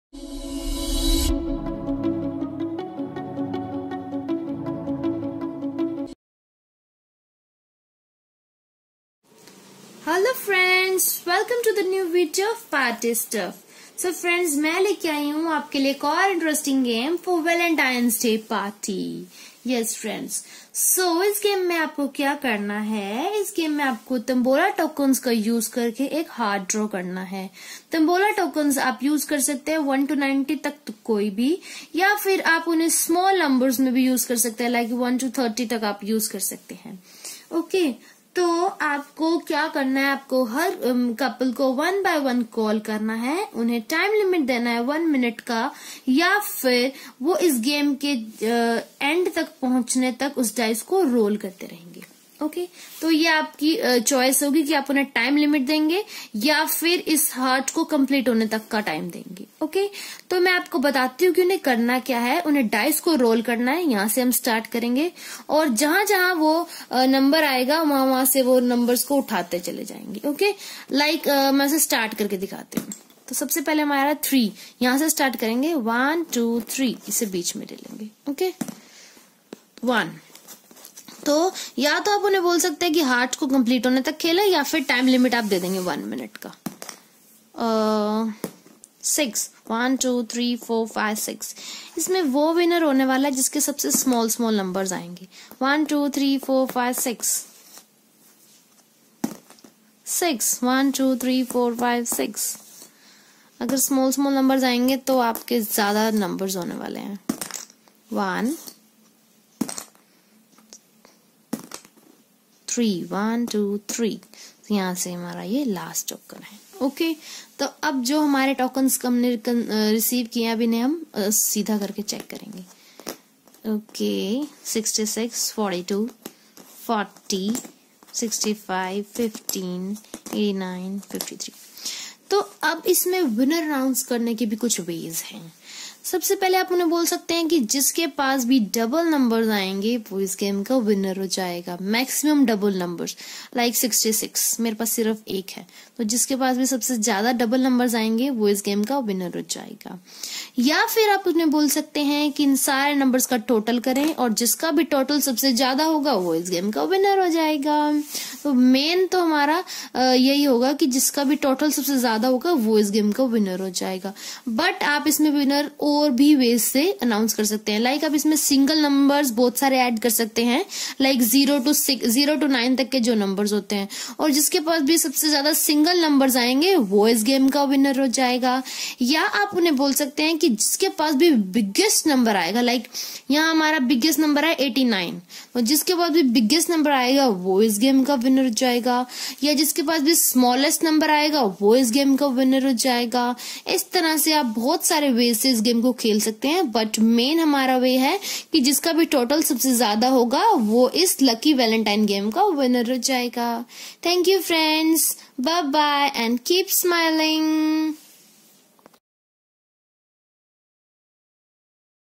Valentine, Romance, Heart - Dice Roll on Tambola Tokens in Heart shape - Kitty Game, Party Game
Dice Roll on Tambola Tokens in Heart shape

Cost - Low
Playing - Easy
Preparation - Easy
Audience: Ladies
Theme: Valentine, Romance, Heart
Occasion: Valentine
Genre: Kitty Game, Party Game